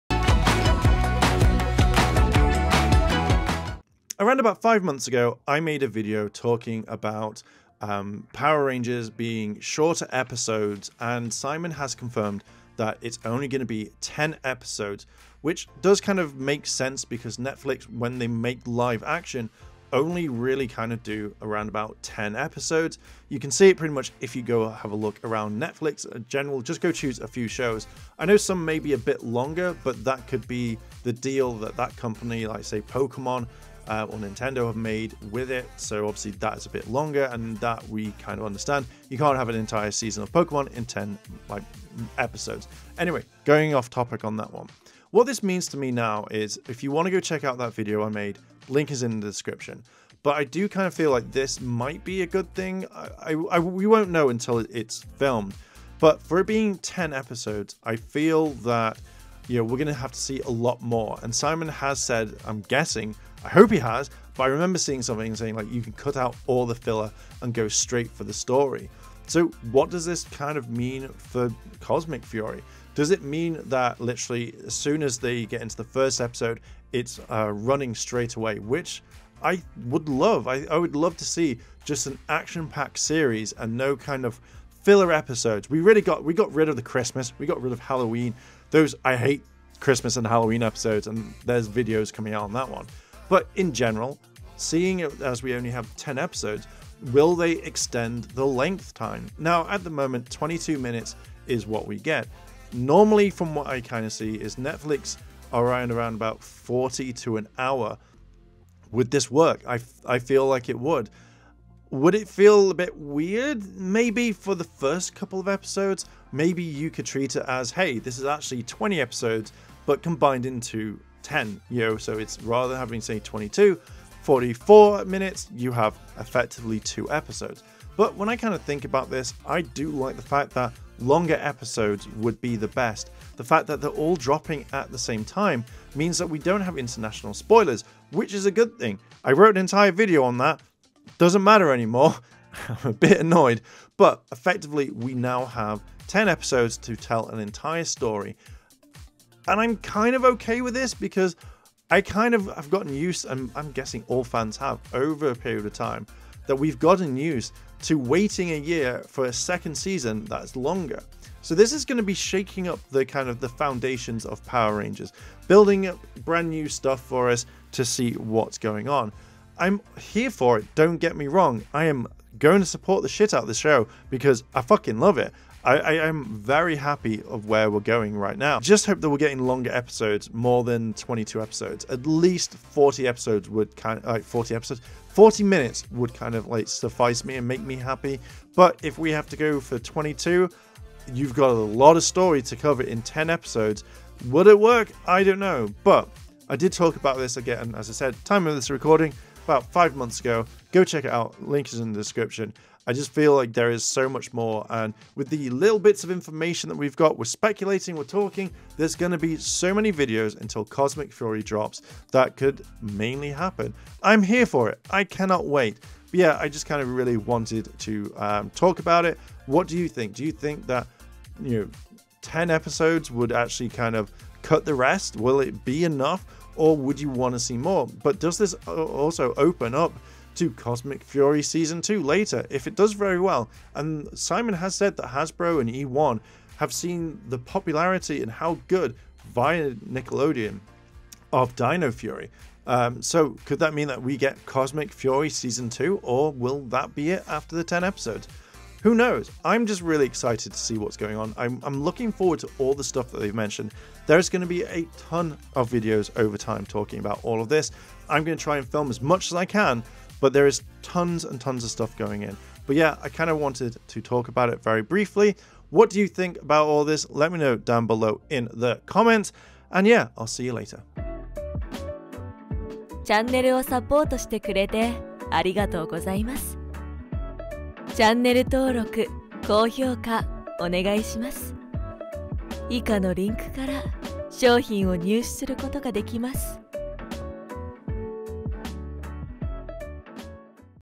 Will 10 Episodes work?
Don't forget to Subscribe to the channel and welcome back to another video, It's interesting as I've spoken about this before around about 5 months ago from this video and went into more depth with it, I thought this might be the case and it came to be true and all. 10 episodes means it will be straight to the point which is fine I think to be honest, I'm now curious on what they are going to do with the series.